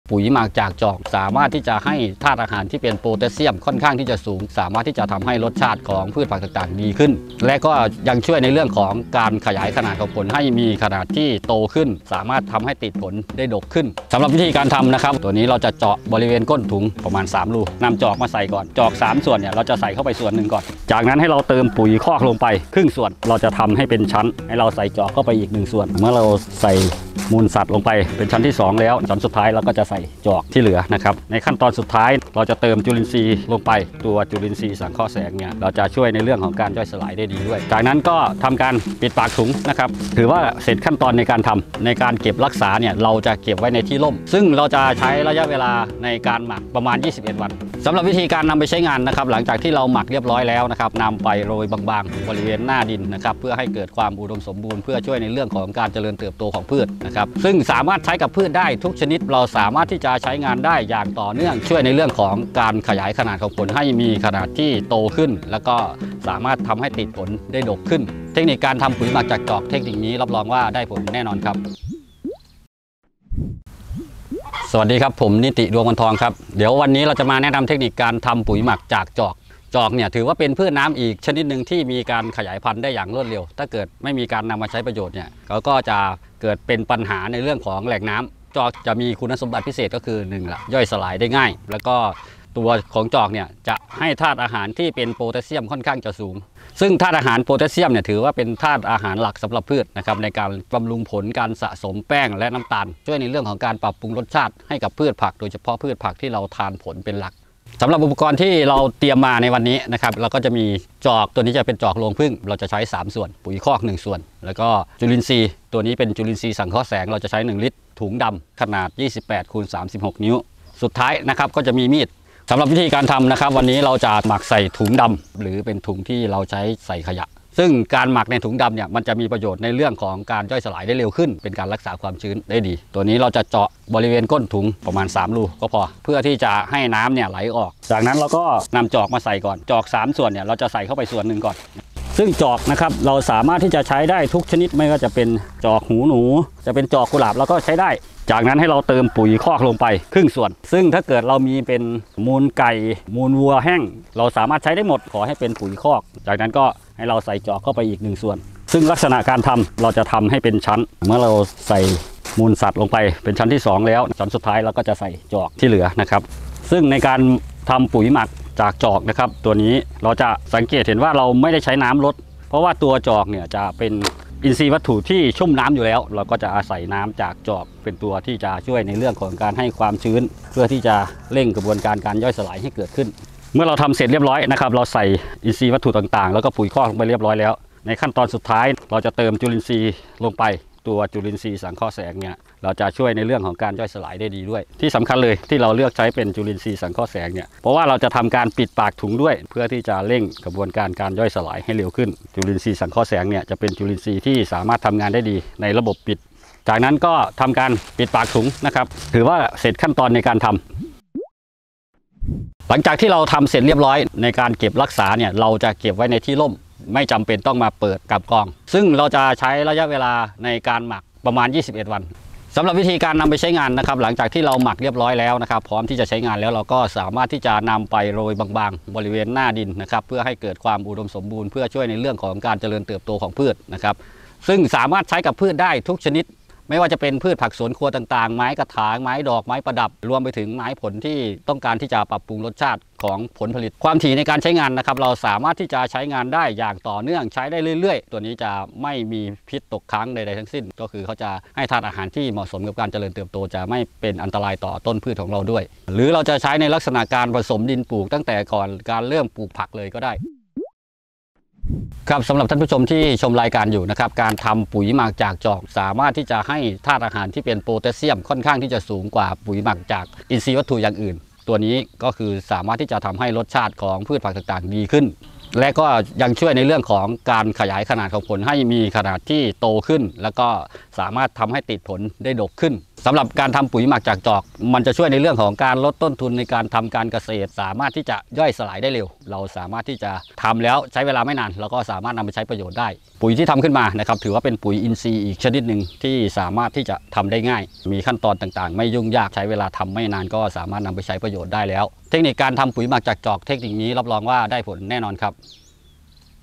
ปุ๋ยหมักจากจอก เสริมธาตุโปแตสเซียมให้แก่ดิน บำรุงพืชงาม
สถานที่ถ่ายทำ : ต.ดอนกลาง อ.โกสุมพิสัย จ.มหาสารคาม